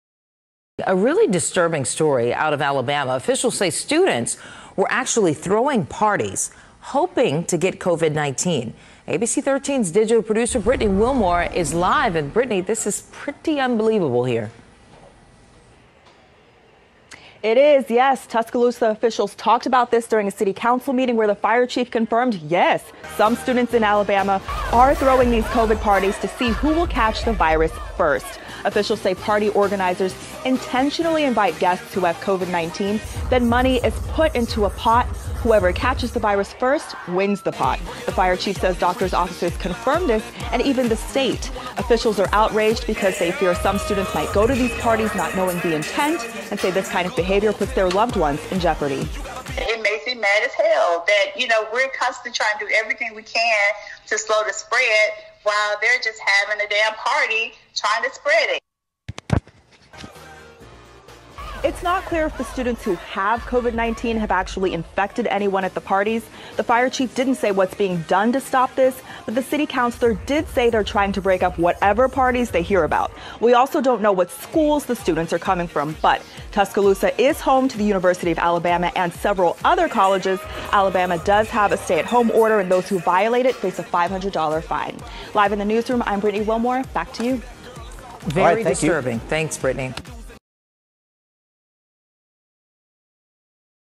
Covid Party Winner in Alabama catches Covid 19
Many who attend "COVID party" are competing to see who can catch the virus first. During events reported in Alabama, some college students even gambled money to give to the "winner," the one who catches COVID-19 first.

Appleby said she is seeing more and more coronavirus patients in their 20s and 30s. She said she shared the story not to scare people, but to make sure they understand that the virus can affect anyone.

"He thought he was young and he was invincible and wouldn't get affected ... One of the things that was heart-wrenching that he said to his nurse was, 'I think I made a mistake,'" she said.